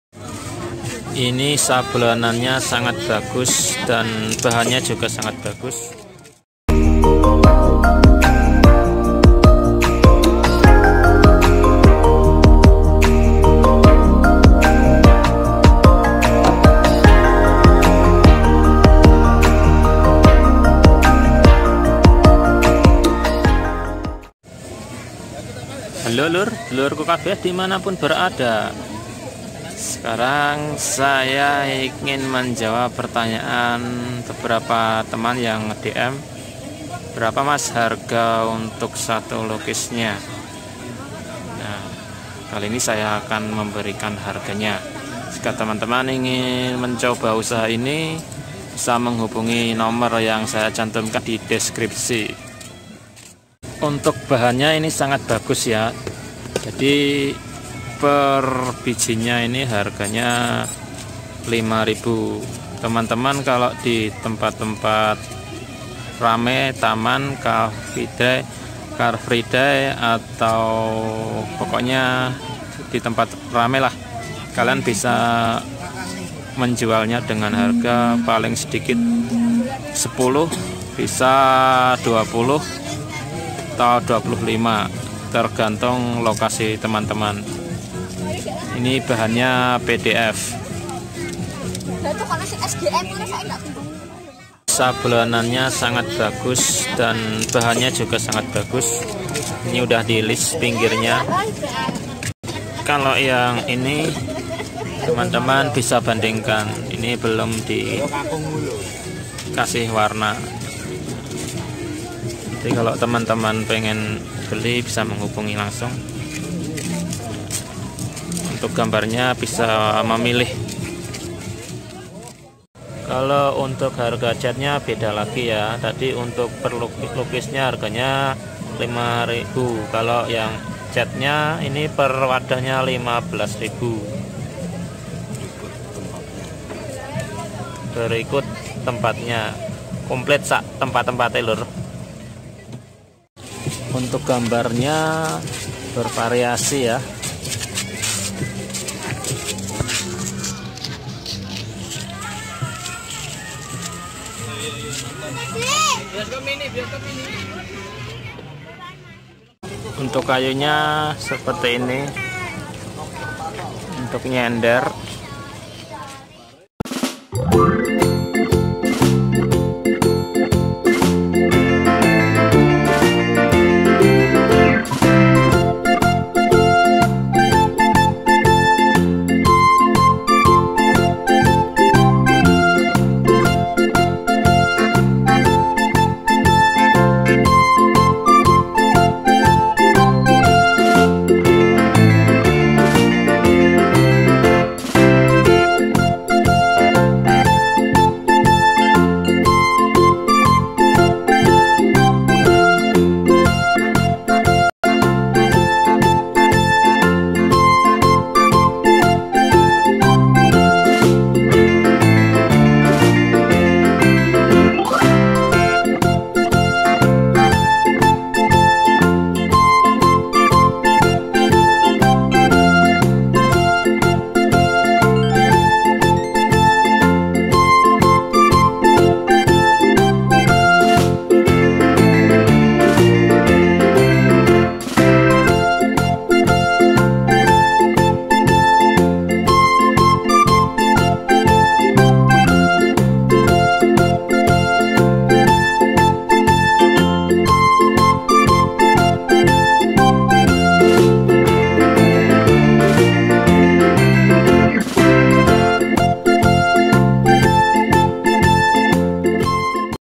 Paket usaha edukasi mewarnai / melukis
Usaha edukasi mewarnai
Bahan Bagus berkualitas!!

harga bisa berubah sewaktu-waktu.

Jangan bandingkan dengan bahan steorofom/ fotocopy an.. Jauh bgt kualitasnya !

disini jual bahan mdf ya.. bukan sterofom

Bisa2 temen2 jadikan ide usaha/ sampingan
Semoga bermanfaat